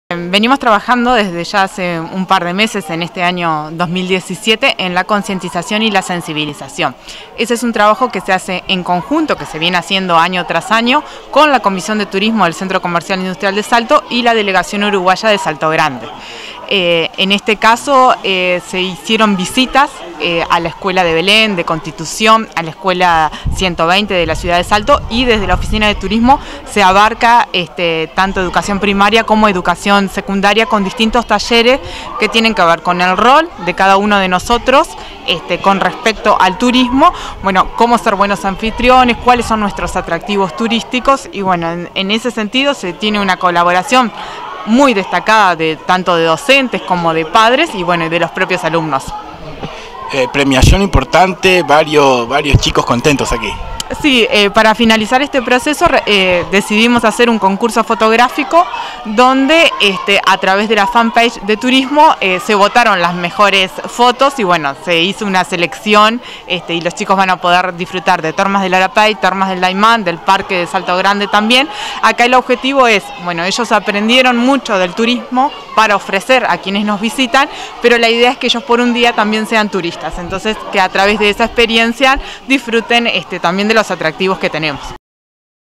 Entregan reconocimiento del concurso Fotográfico “Descubriendo Salto”
Se llevó a cabo el cierre de los talleres de capacitación turística dirigidos a escolares, organizados por la Comisión de Turismo del Centro Comercial, la Intendencia y CTM, con un acto de premiación a las escuelas que participaron del Concurso Fotográfico “Descubriendo Salto”. Los talleres se implementaron durante el mes de junio y estuvieron dirigidos a las escuelas públicas ubicadas en los principales centros turísticos, con el Concurso Fotográfico como cierre.